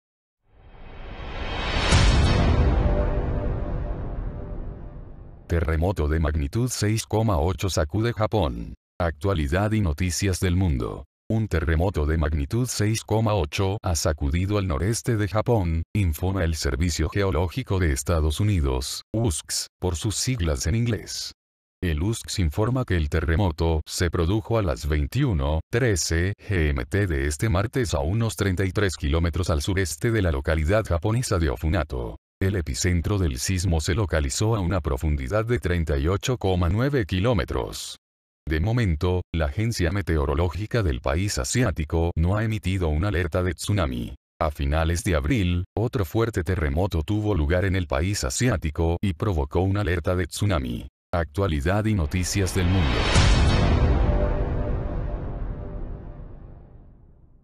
Terremoto de magnitud 6,8 sacude Japón.
Un sismo de magnitud 5,1 se registra en Indonesia.

Un terremoto de magnitud 6,8 ha sacudido el noreste de Japón, infoma el Servicio Geológico de EE.UU. (USGS, por sus siglas en inglés). Vicita nuestra paguna de recursos gratuitos